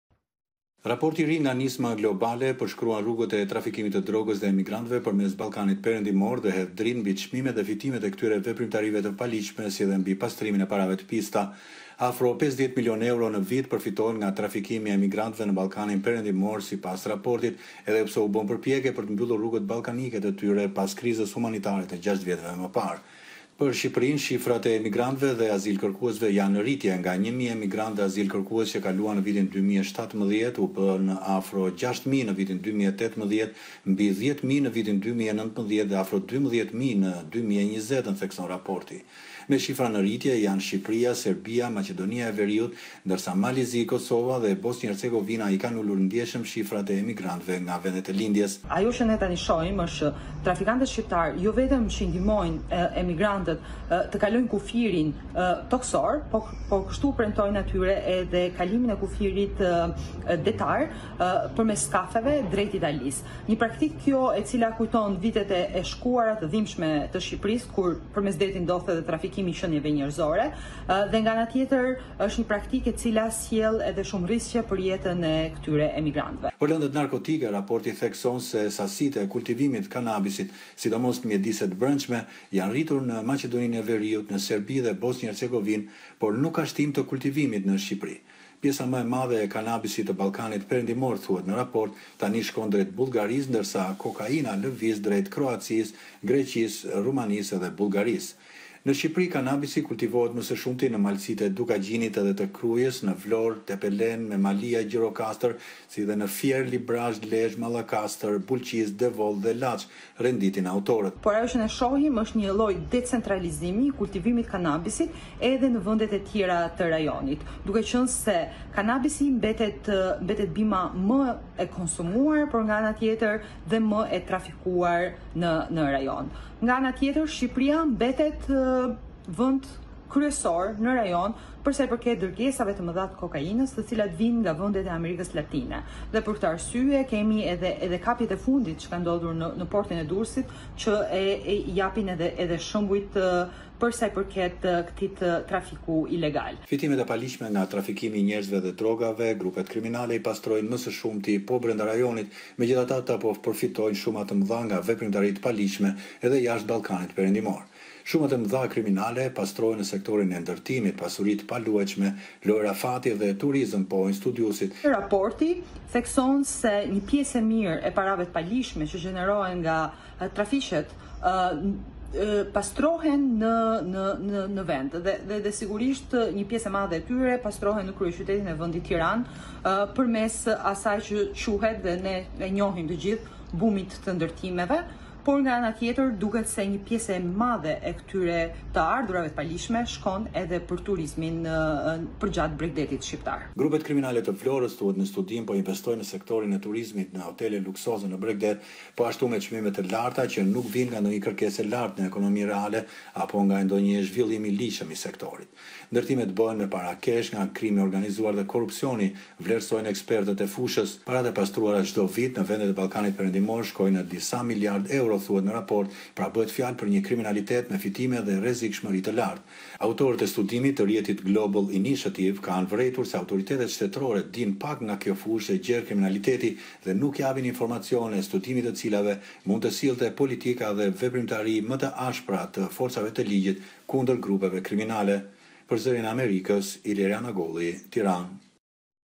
Raporti: Ballkani Perëndimor, një udhëkryq i krimit të organizuar, drogave dhe parave të pista
Rrjeti “Nisma Globale Kundër Krimit të Organizuar Ndërkombëtar” publikoi sot një raport të ri mbi flukset e emigrantëve, drogës dhe parave të pista në Ballkanin Perëndimor. Raporti tregon për një veprimtari të shtuar të grupeve kriminale në këtë rajon me trafikimin e azilkërkuesve dhe drogave, si dhe me pastrimin e parave përmes ndërtimit dhe lojërave të fatit.